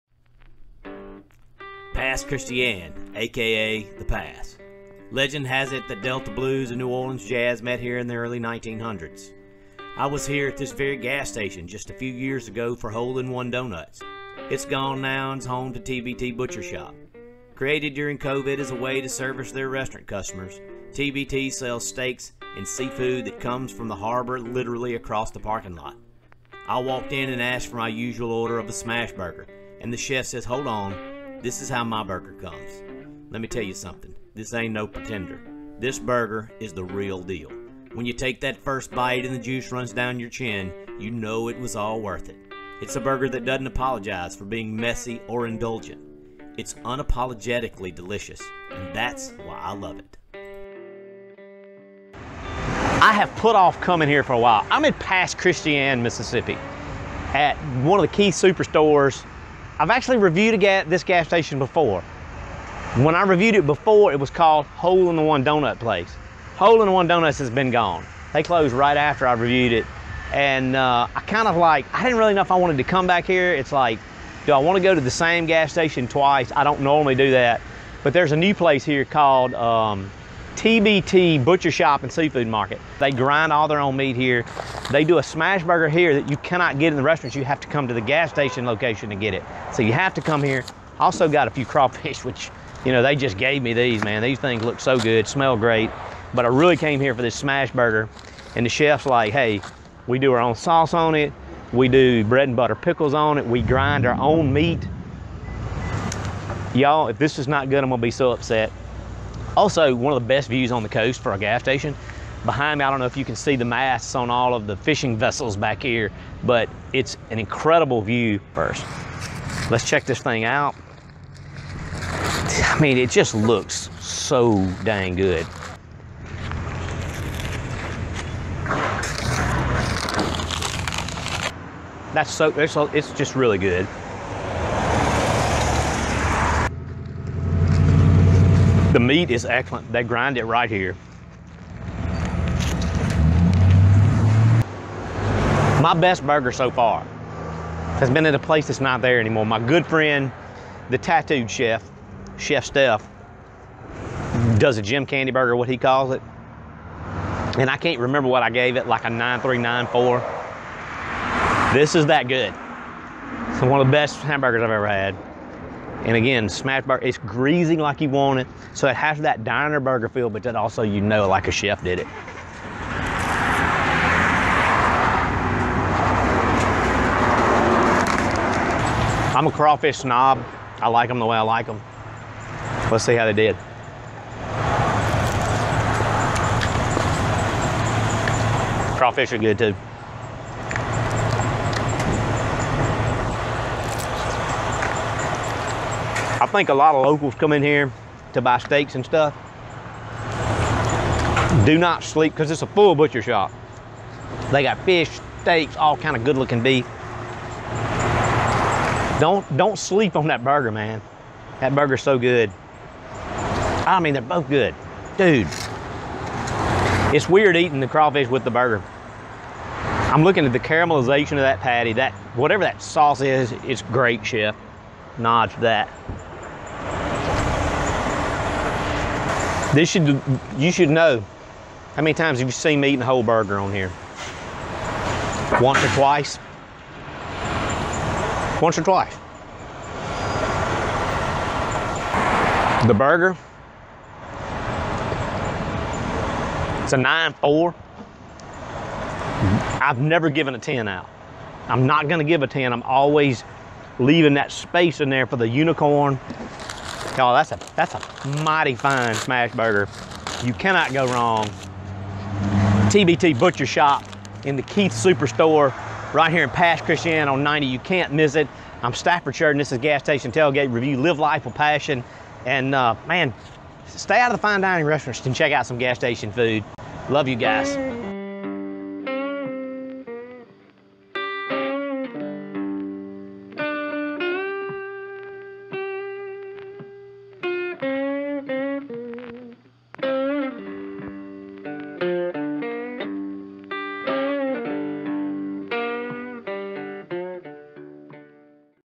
Ep 142, TBT Butcher Shop, Pass Christian, MS
This used to be hole in one donuts but now they are TBT Butcher Shop. You can't miss the gas station on the marina with the great smash burger!